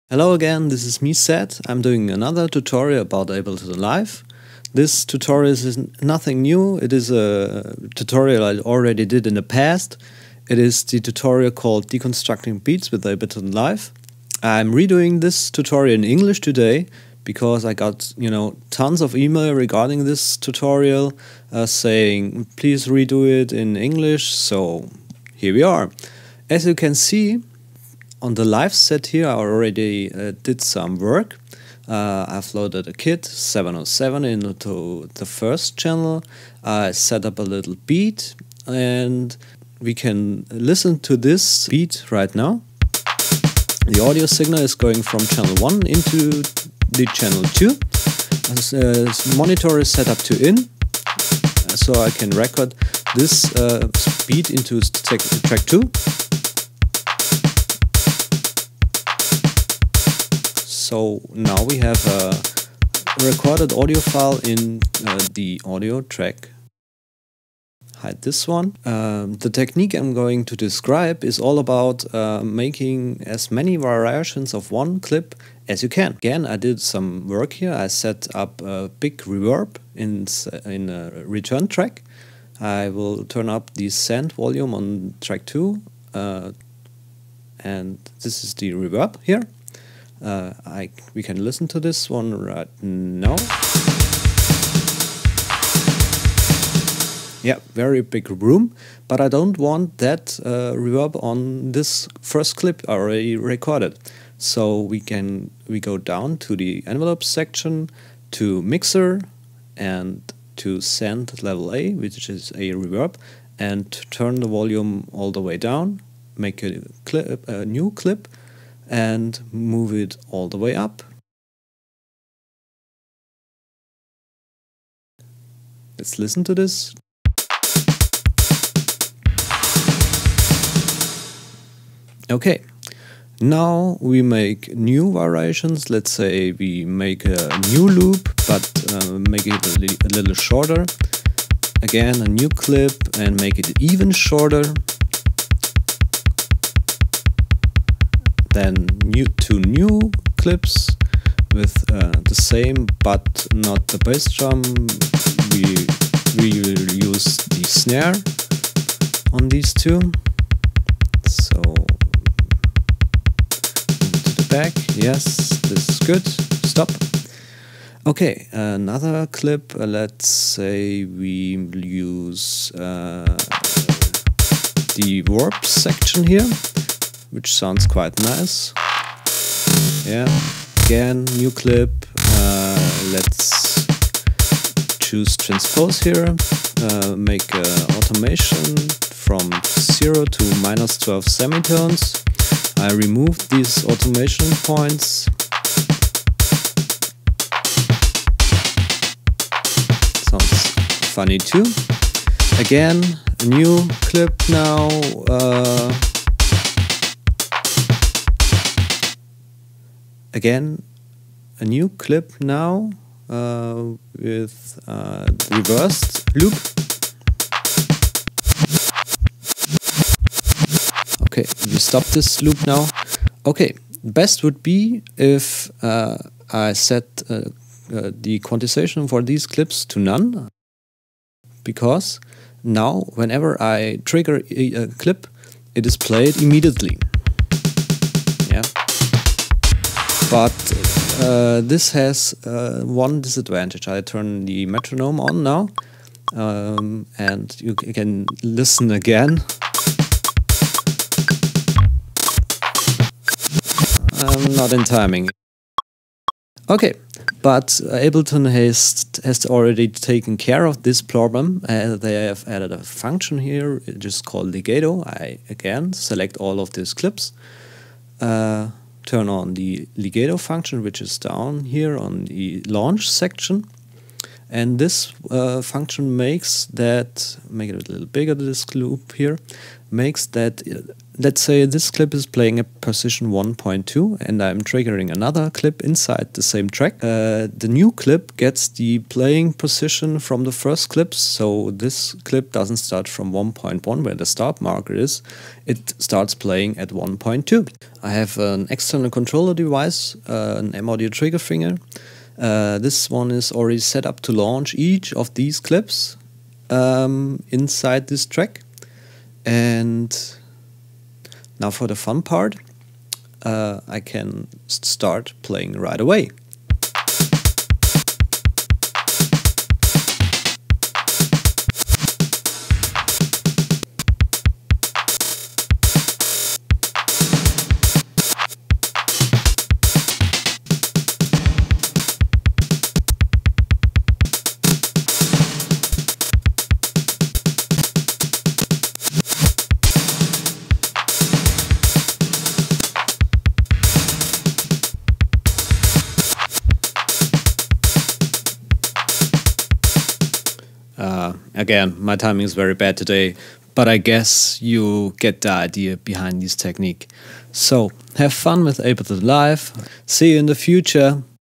Deconstructing Beats with Ableton Live [english] (by Zettt)
Use Legato function in Live to make cool beats.
How to:
- Make a clip
- Make variations of this one. (as many as you can)
- Set Quantization to none and turn on legato for these clips
- Have fun!